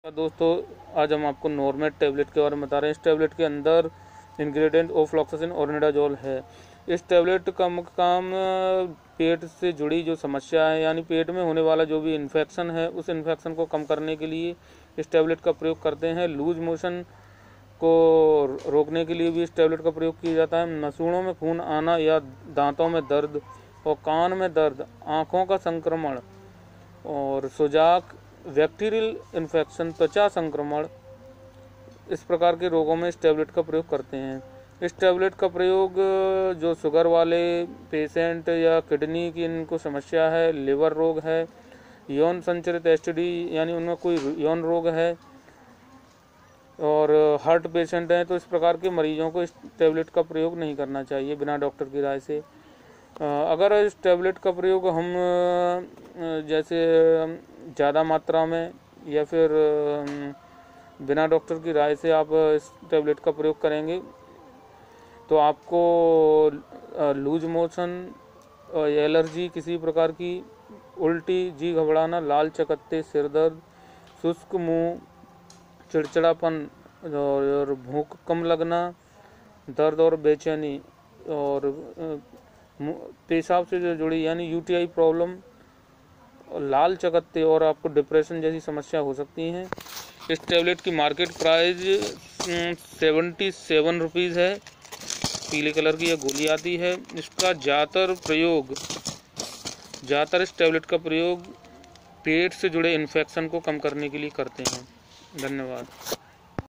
NORMET Tablet Rx_ ofloxacin & Ornidazole
Normet tab
              यह एक antibiotic tablet है । ज्यादातर इस टैबलेट का प्रयोग पेट में होने वाले infection को कम करने के लिये किया जाता है ।
     Side effect- दर्द और बेचैनी , लाल चकत्ते , उल्टी , जी घबराना , जलन , मूत्र विकार , सिर दर्द , ठण्ड लगना , चक्कर आना आदि समस्या हो सकती हैं ।